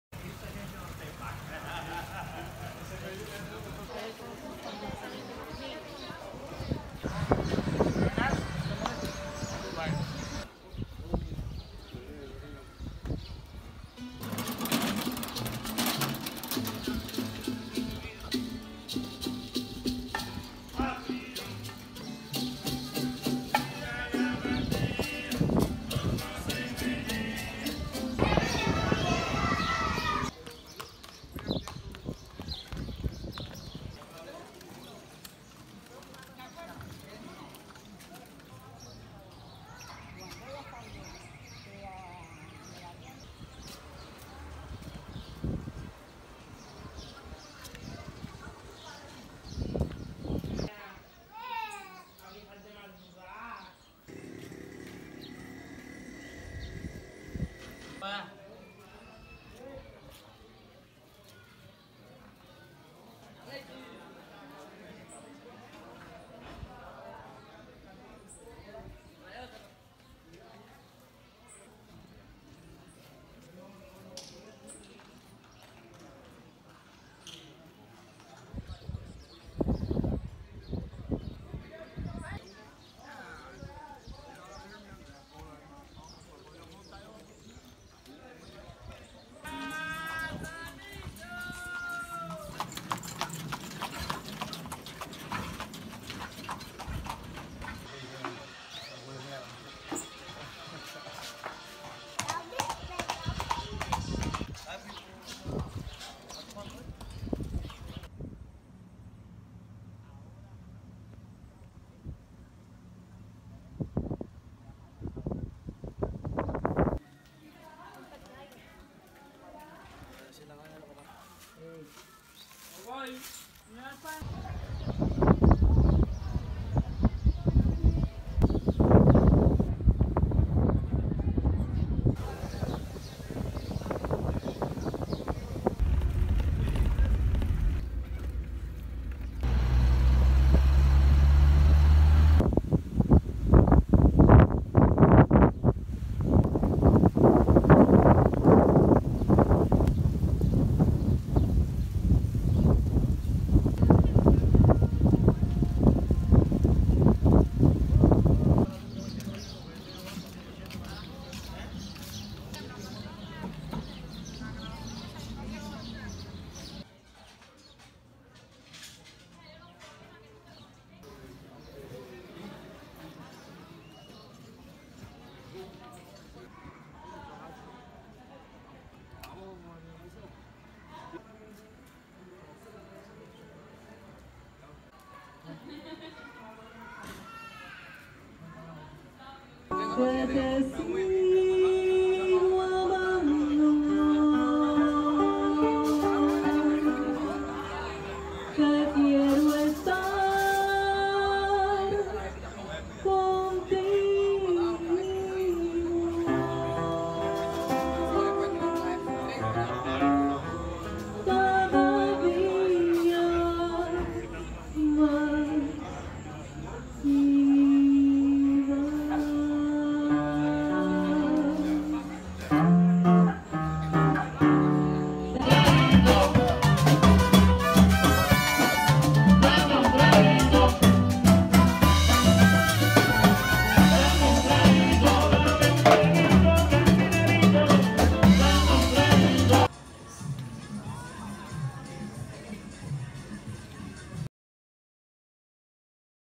A day in Trinidad de Cuba
Touring the UNESCO world heritage city, from the Plaza Mayor to top of radio tower hill, and from the white sands of Ancón beach to the casa de la musica, where everyone salsas the night away.